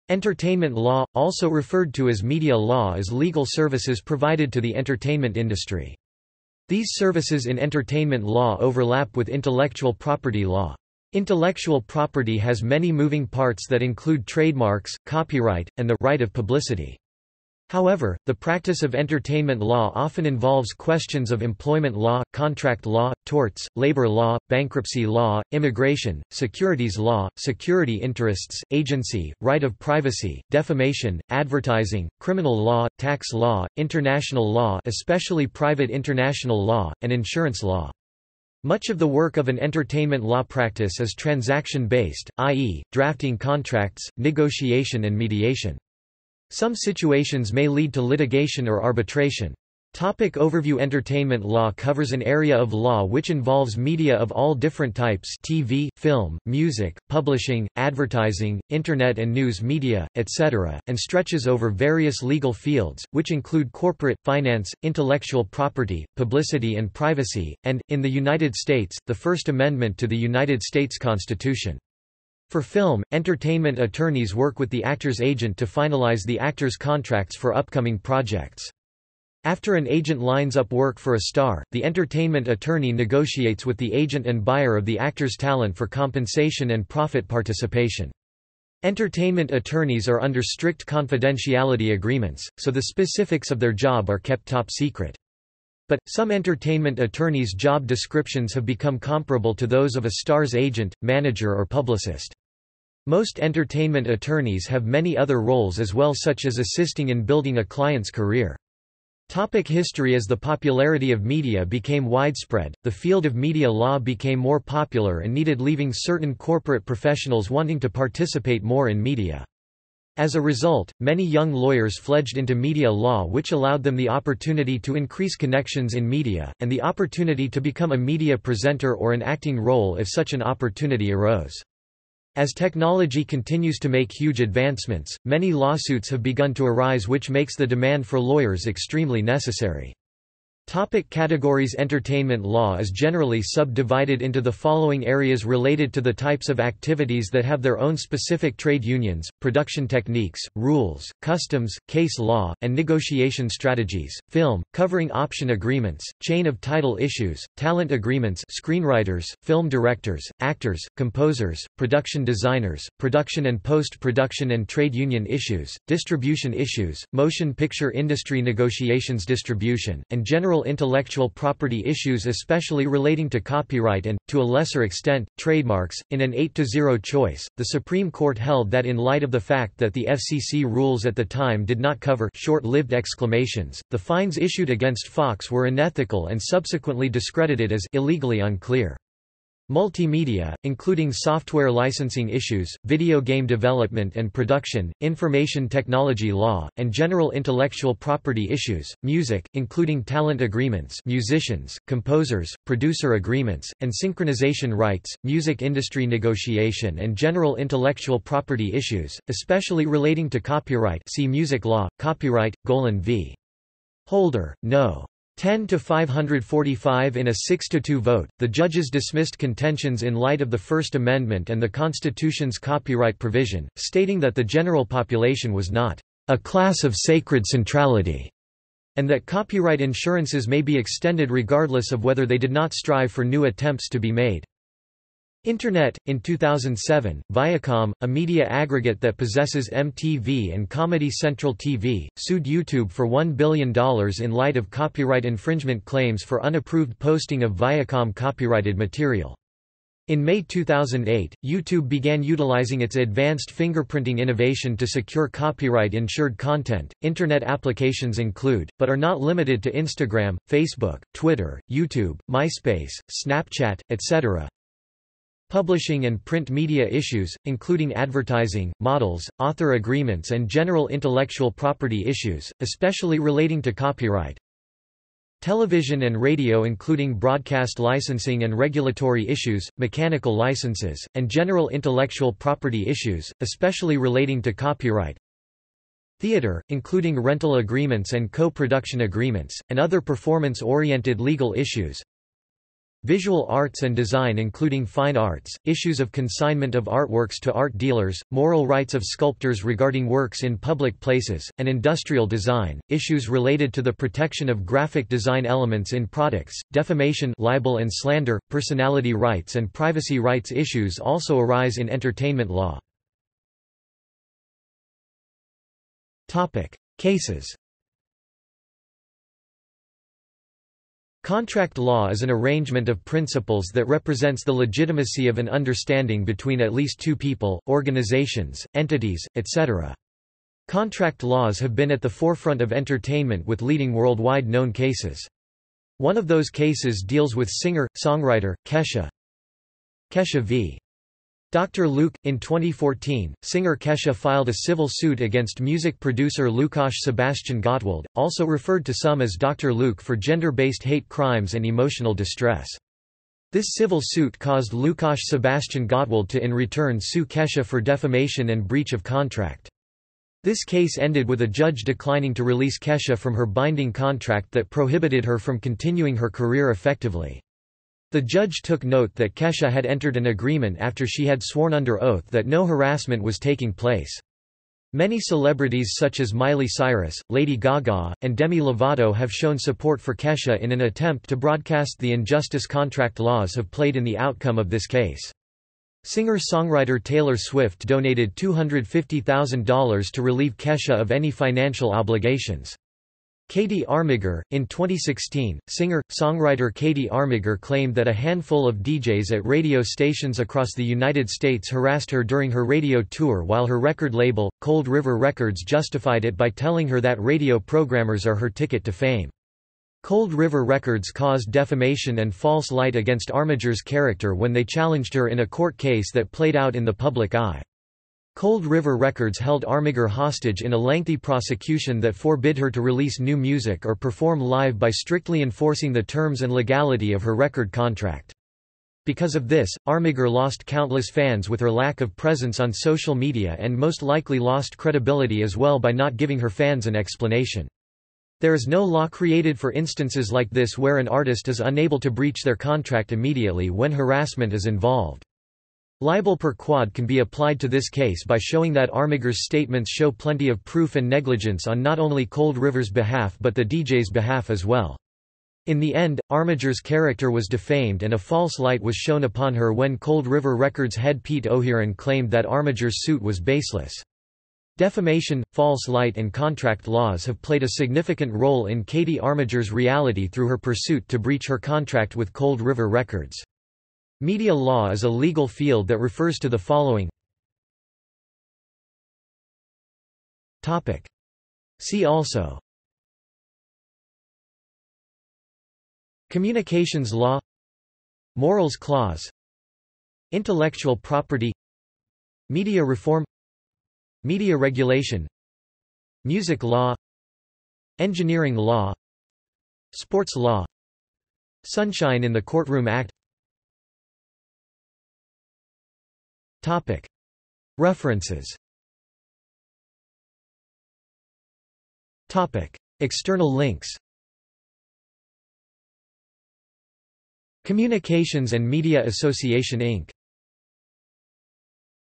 Entertainment law | Wikipedia audio article
This is an audio version of the Wikipedia Article:
Entertainment law

- Socrates

SUMMARY
Entertainment law, also referred to as media law is legal services provided to the entertainment industry. These services in entertainment law overlap with intellectual property law. Intellectual property has many moving parts that include trademarks, copyright, and the "Right of Publicity". However, the practice of entertainment law often involves questions of employment law, contract law, torts, labor law, bankruptcy law, immigration, securities law, security interests, agency, right of privacy, defamation, advertising,  criminal law, tax law, International law (especially Private international law), and insurance law.
Much of the work of an entertainment law practice is transaction based, i.e., drafting contracts, negotiation and mediation. Some situations may lead to litigation or arbitration.